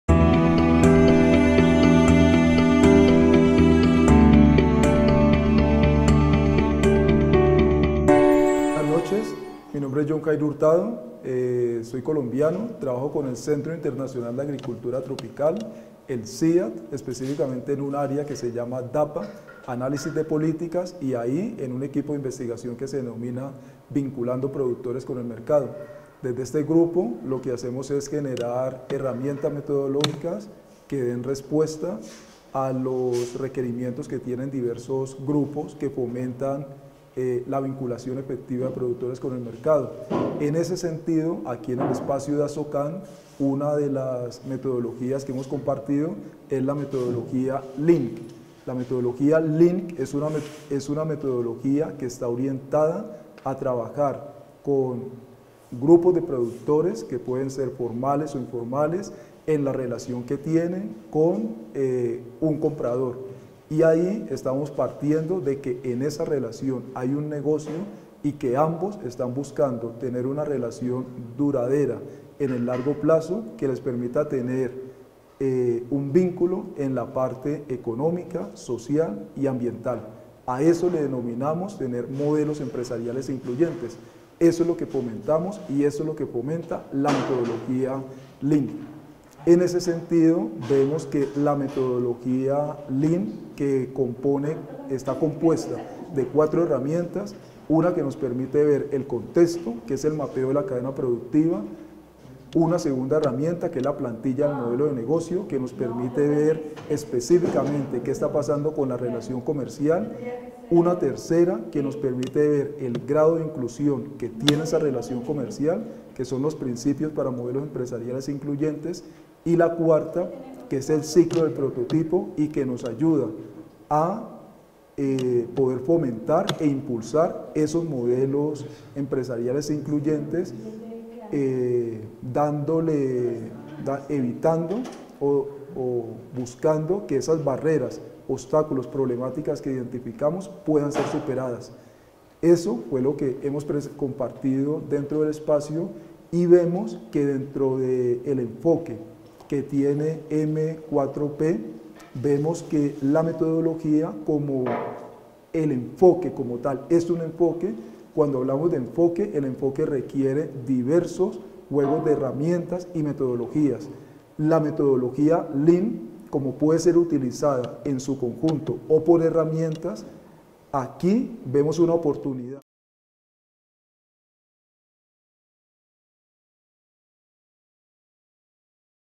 Jhon Jairo Hurtado - Experiencia CIAT Colombia
Jhon Jairo Hurtado nos cuenta la experiencia del Centro Internacional de Agricultura Tropical en Colombia, con la aplicación del enfoque M4P